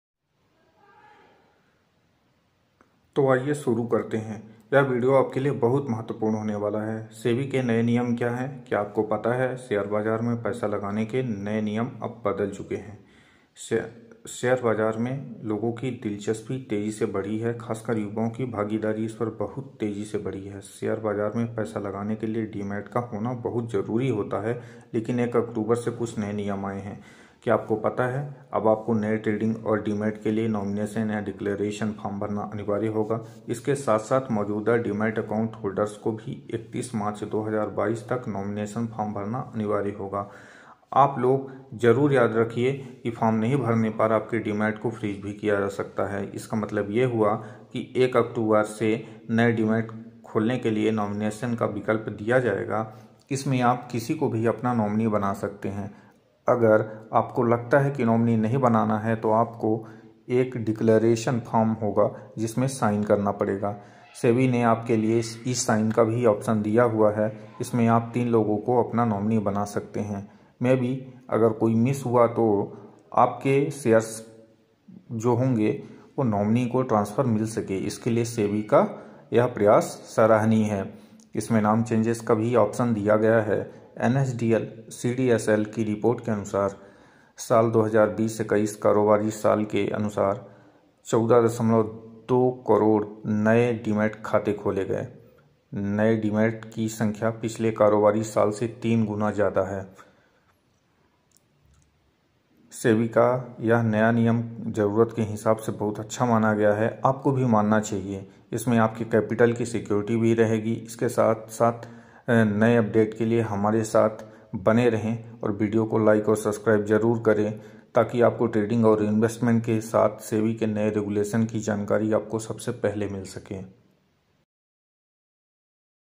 SEBI NE FIR SE BADALA NIYAM || KYA HAIN SEBI KE NAYE NIYAM.??
Read on to find out more about the new rule, what is meant by spoofing and how it affects you.

What Is The New Rule?

From April 5 onwards, stock market traders who try to modify their orders repeatedly without really placing the orders will face the Sebi axe. Their accounts will be disabled for 15 minutes to 2 hours immediately on detection of the violation.

The duration of the ban will depend on the extent of the violation.